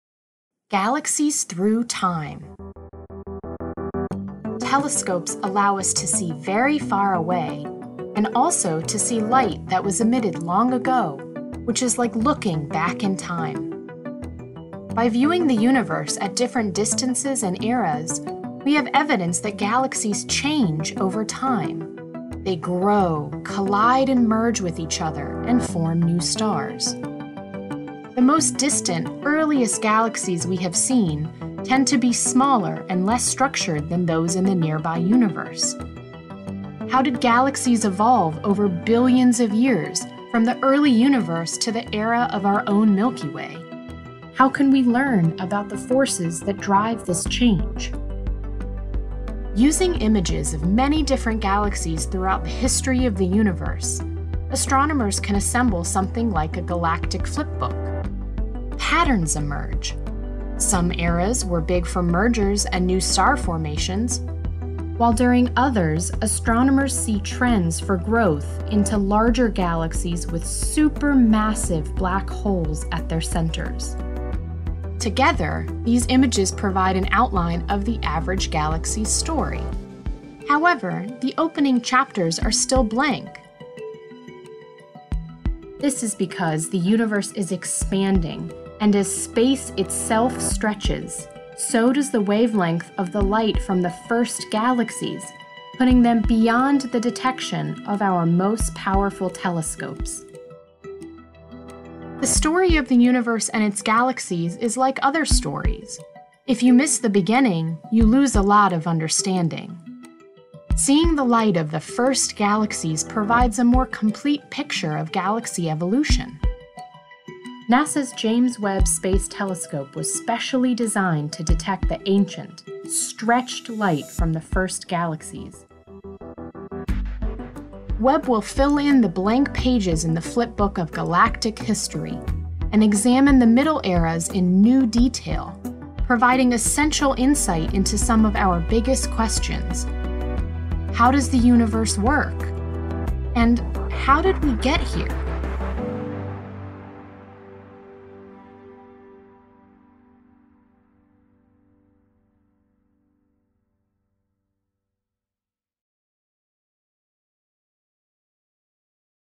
Galaxies Through Time: Discover How to Look Back in Time and Study the History of the Universe
Discover how telescopes make it possible to look back in time and study the history of the universe, and how NASA’s James Webb Space Telescope will fill in new details on galaxy evolution over time. The earliest pages of cosmic history are blank, but Webb will allow us to look back farther in time than ever before, helping to fill in the lost pages of the universe’s story.

Credit: NASA, ESA, CSA, and L. Hustak and D. Player (STScI)